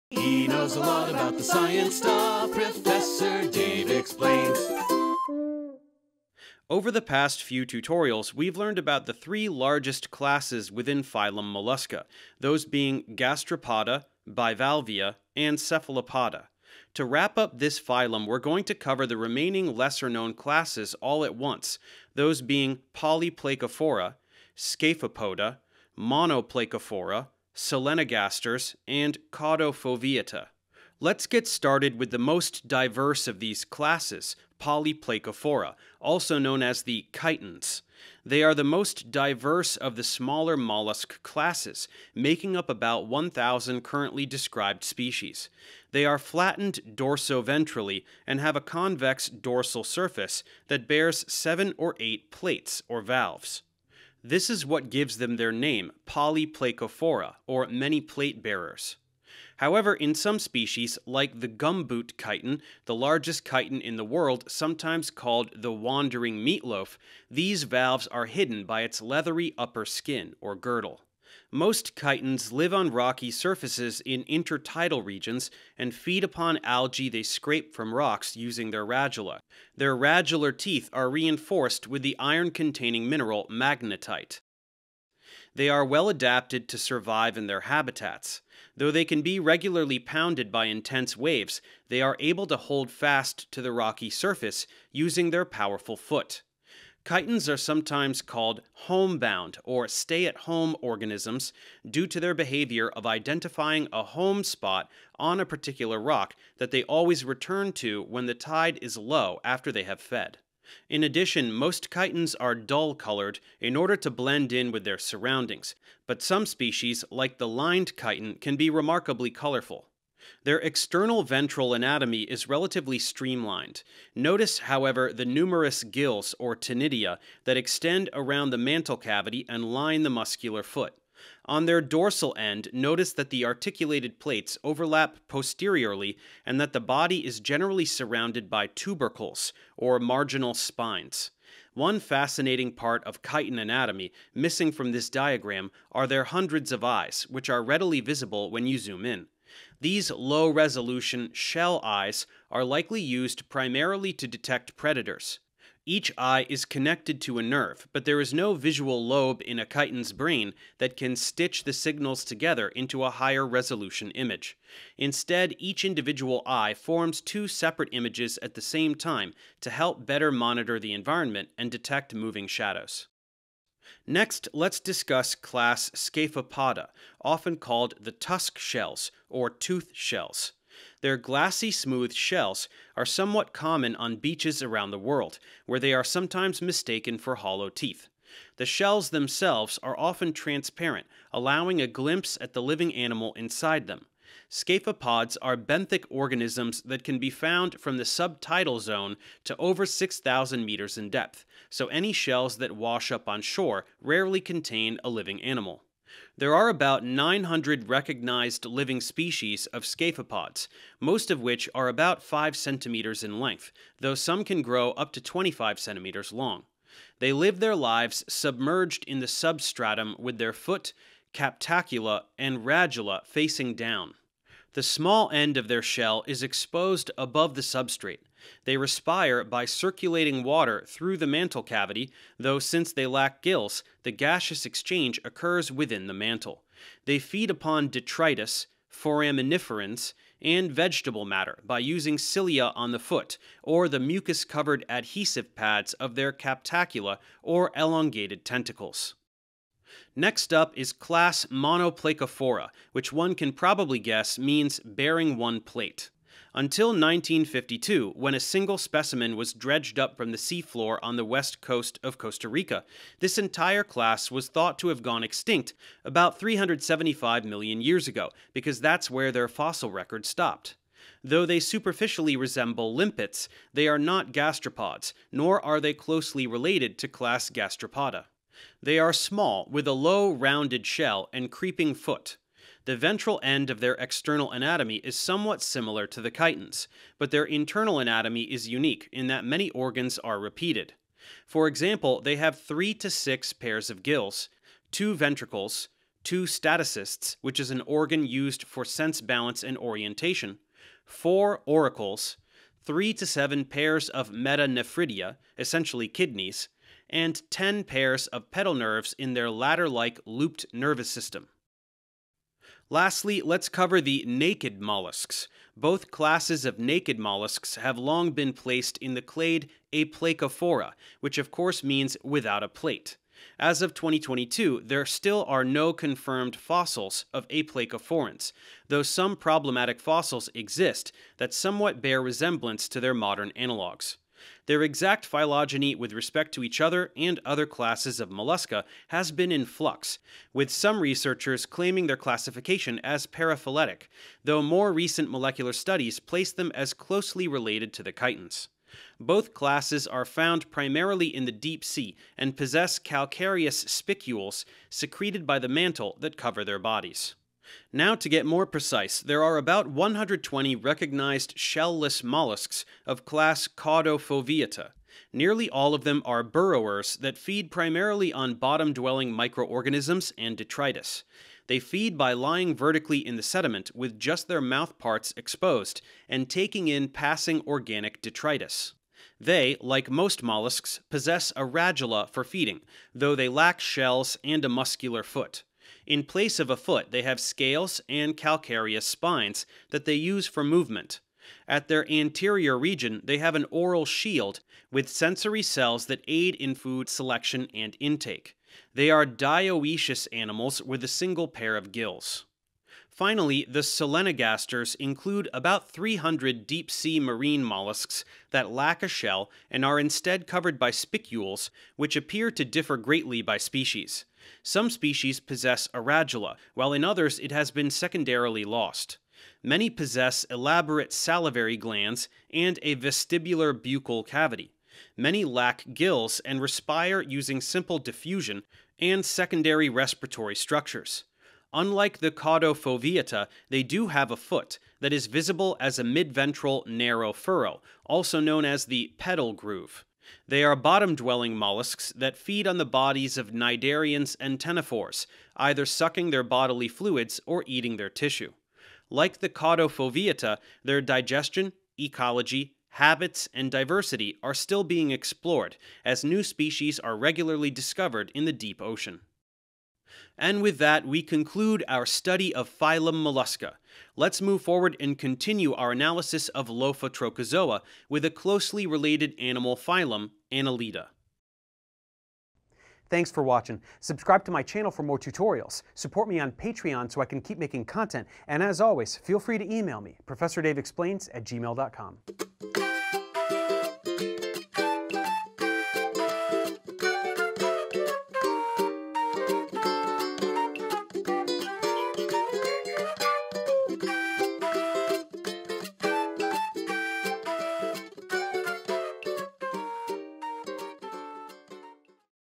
Phylum Mollusca Part 5: The Lesser Known Classes
It's time to wrap things up with the mollusks, so let's look at five smaller classes consisting of lesser known species. These are Polyplacophora, Monoplacophora, Scaphopoda, Solenogastres, and Caudofoveata. What kinds of creatures are these? Let's find out!